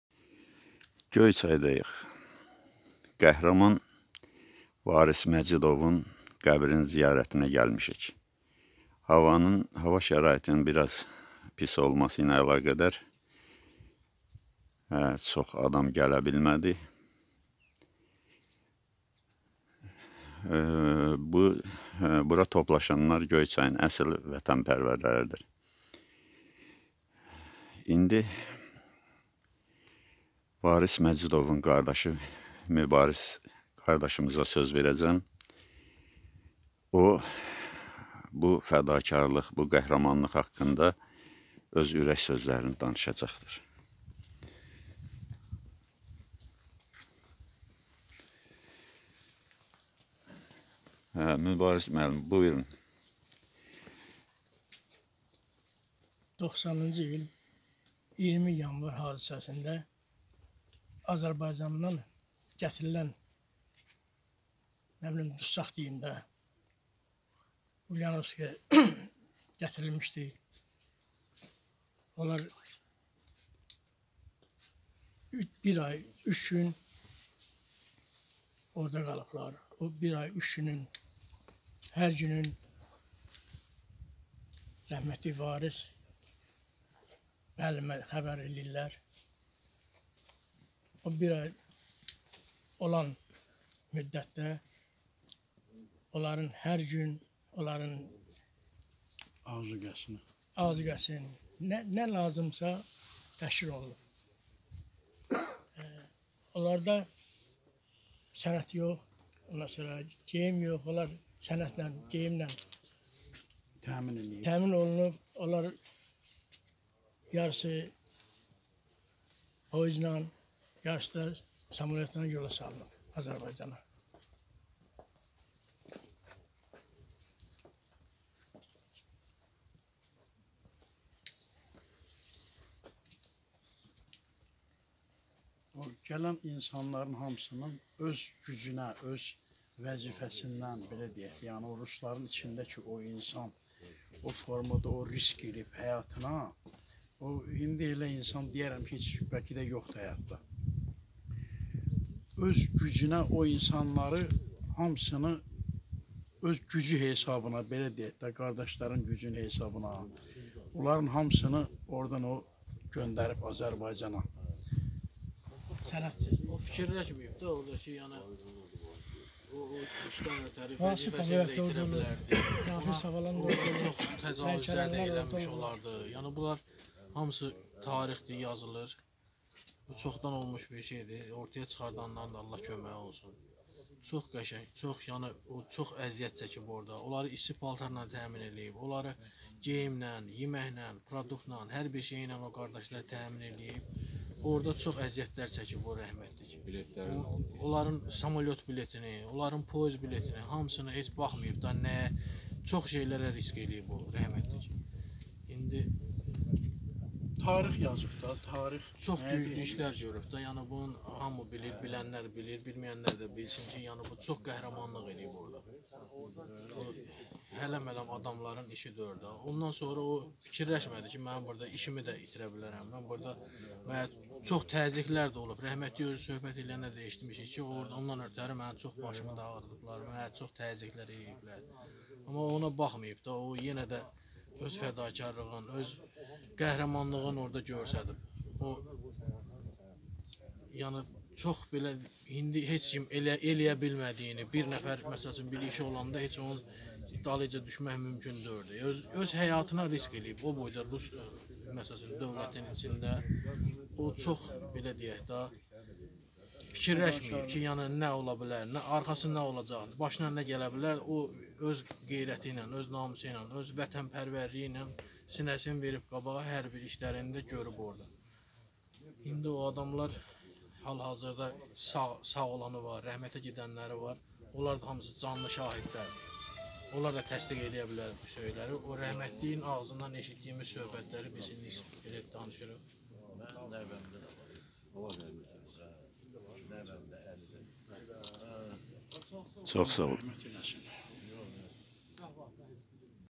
VARİS MƏCİDOV- Yada salınan qəhrəman. 1
Varis Məcidov 90-cı ildə Rusiyanın Ulyanovski şəhərində 46 Azərbaycanlı məhbusu ölümdən xilas etmişdi.